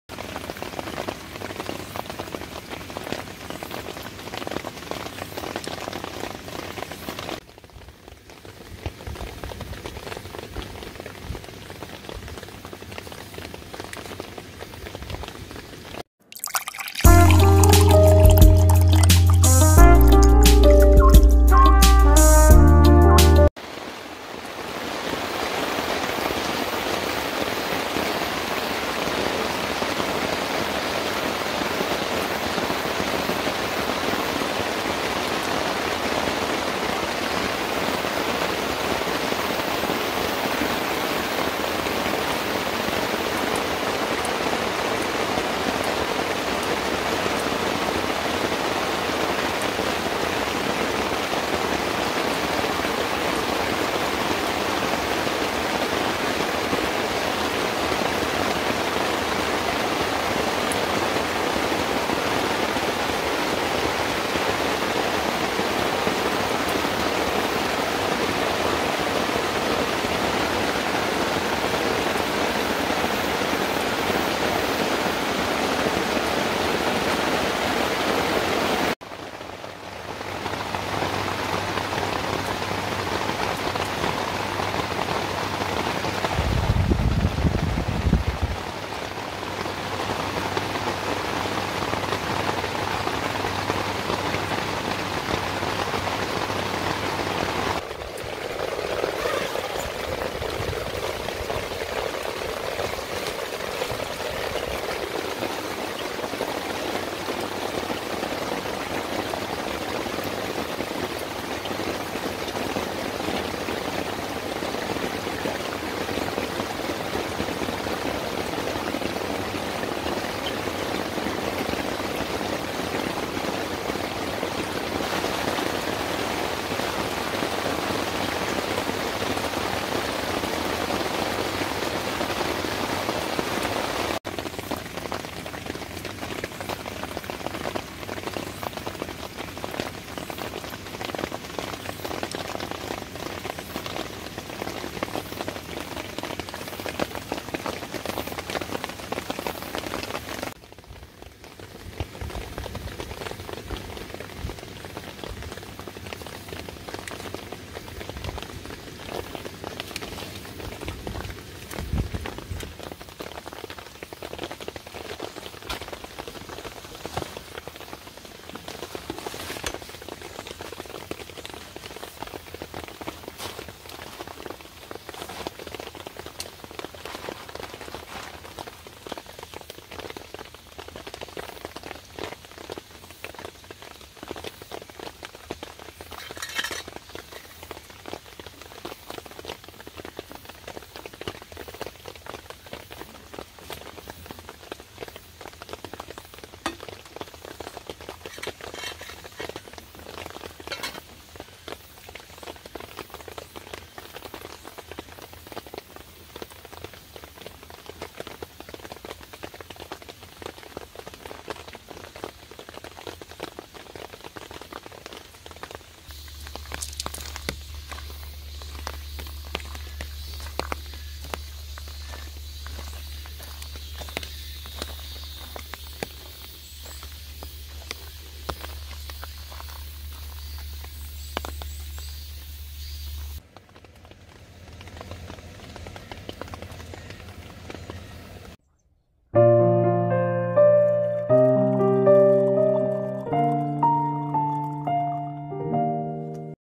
[Korea camping traveler ~ 우중캠핑]2박 3일 캠핑하는 동안 빗소리만 들은 역대급 우중캠,Raining in Camping for 3 days 2 nights
2박 3일 캠핑하는 동안 빗소리만 들었다
솔캠의 고독한 캠핑 이야기